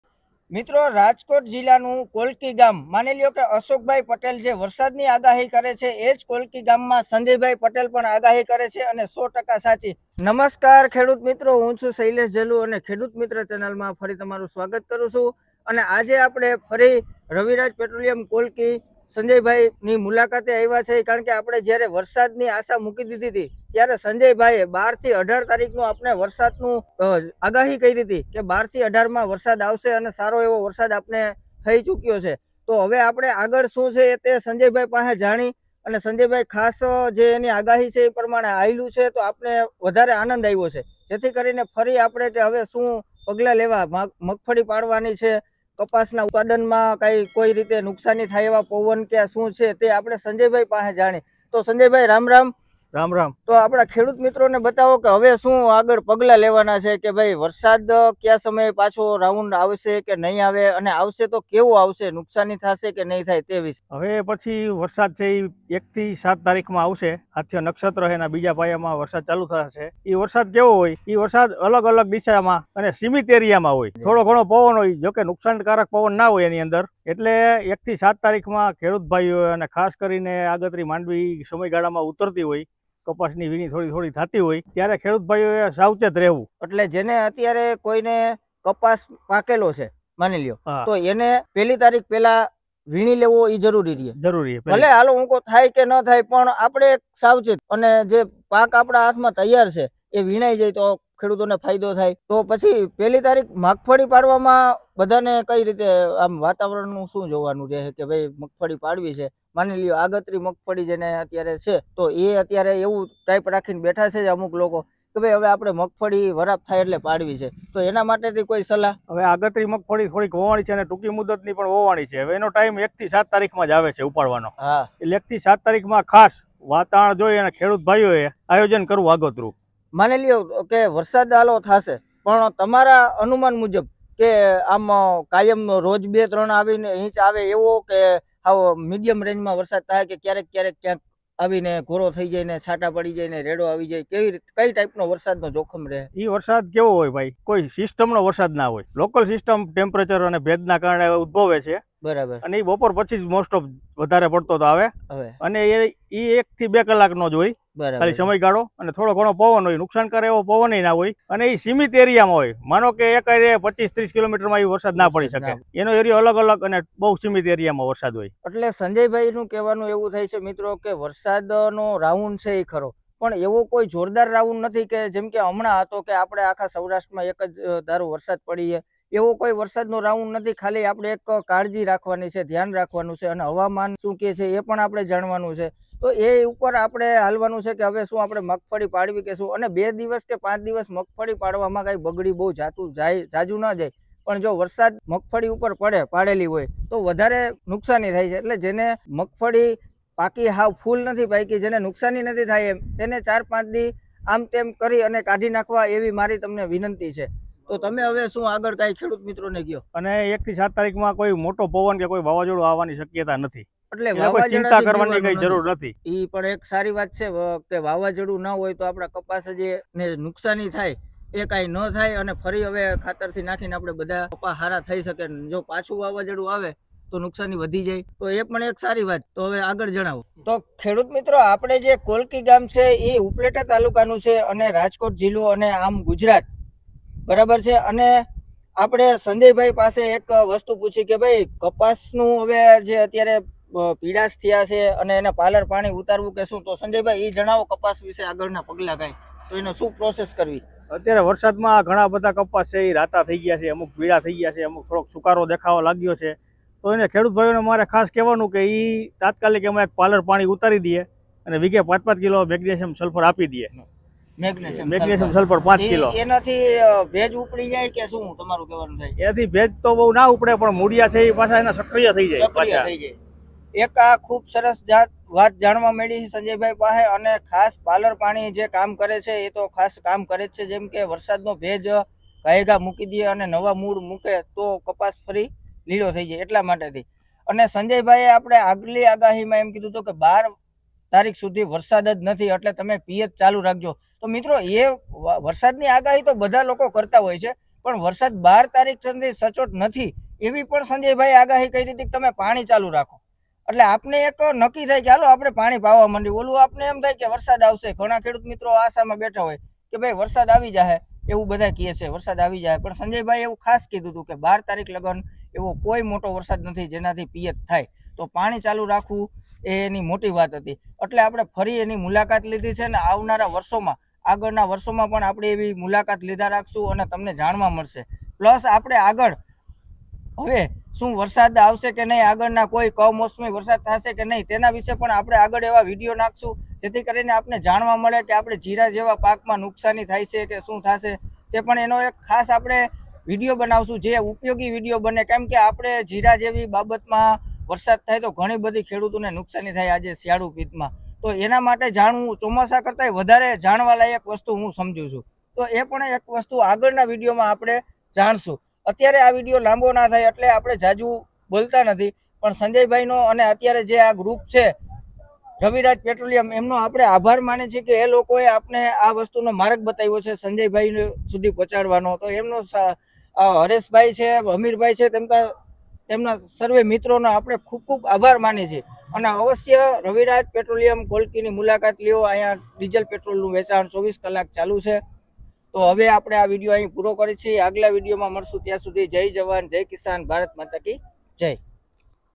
વરસાદ ના નવા રાઉન્ડ ની આગાહી | rain forecast | monsoon 2023 (@khedutmitra6210 )
મગફળી પાડતા પહેલા આ વિડીયો અવશ્ય જુઓ ફરીવાર આવી શકે છે ગુજરાતમાં વરસાદ જો આ વિડીયો પસંદ આવે તો લાઈક કરો શેર કરો અને આપણી ખેડૂત મિત્ર ચેનલ ને સબસ્ક્રાઇબ કરો

            🙏🙏🙏🙏🙏ધન્યવાદ 🙏🙏🙏🙏🙏